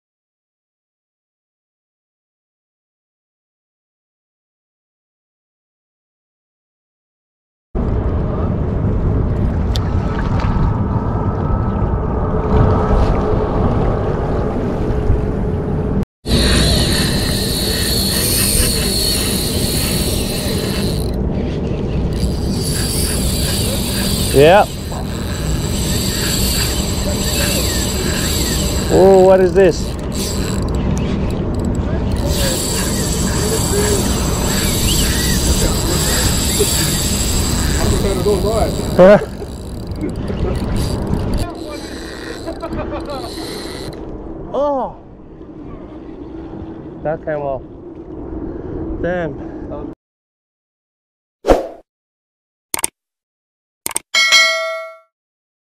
◉[SHORT]◉ Daiwa Exist 2022 screaming reel
Testing out new Daiwa Exist 2022 in LT3000D. Didn’t expect a good size kingy/mulloway would take it in the first day. The reel smooth as, drag sound is so crisp and just what you’d expect for this exquisite reel.

Unfortunately the fish wrapped me around the kelps and took off with my Daiwa Samurai jig 20g.

Rod = Palm Shoregun SFTGS-811L / BL
Reel = Daiwa Exist LT3000D
Main line = Sunline Siglon x8 16lb
Leader = Sunline FC Rock 16lb